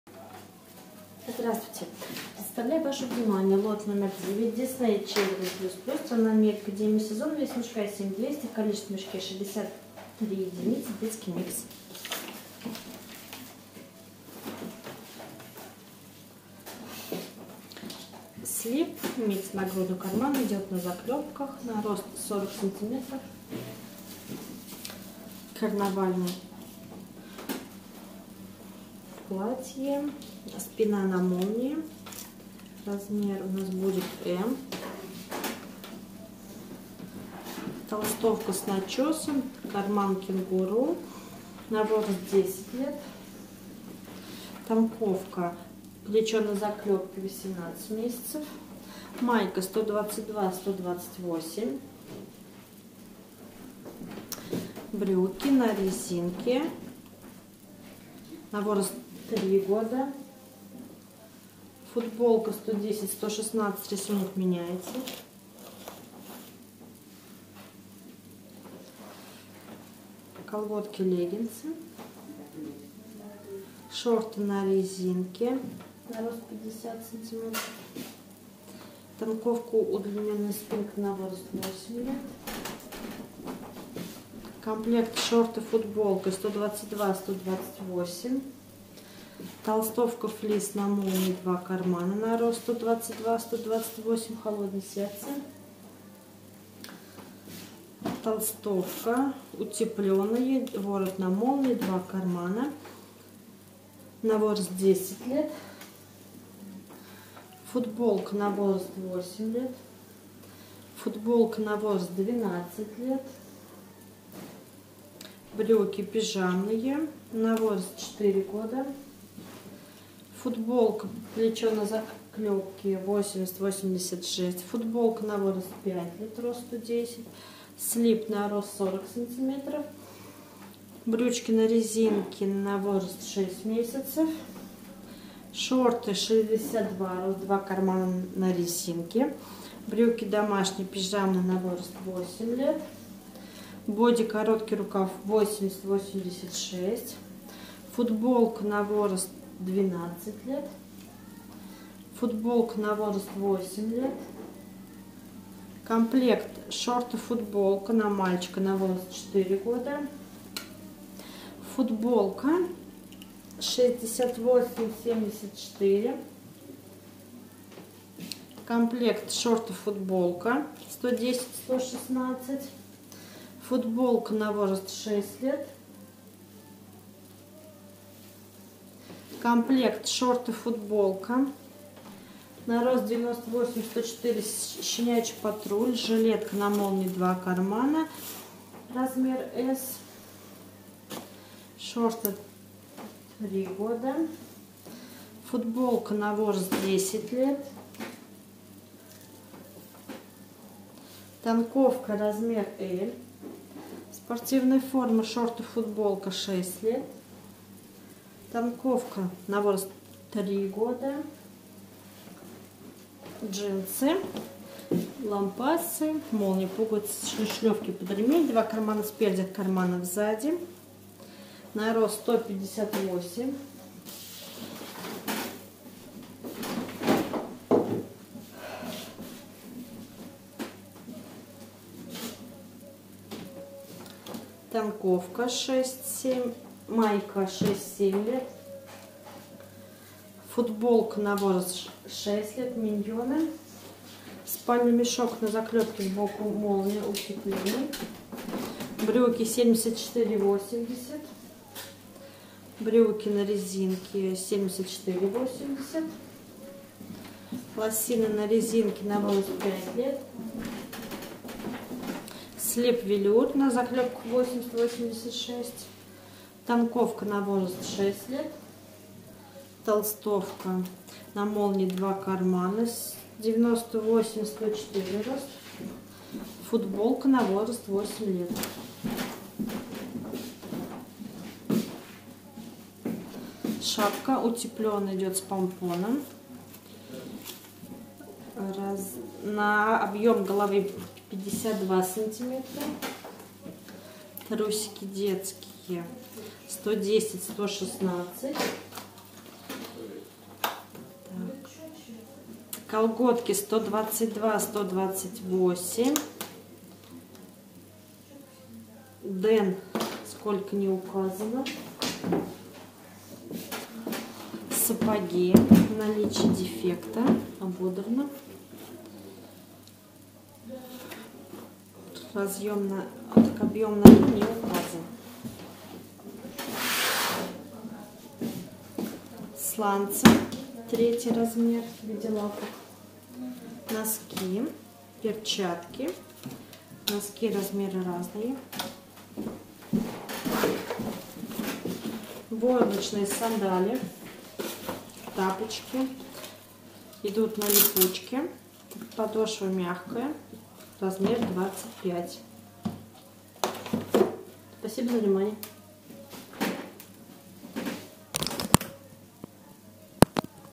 Disney childrens mix ++ 9, Цена : 20.58 евро за кг, сток одежда оптом
Disney childrens mix ++  (Лот№ 9 )
Детский микс (сток)
Страна: Америка
Демисезон
Цена за кг.:  20.58 евро
Цена за мешок: 148.18 евро
Вес мешка, кг.: 7.2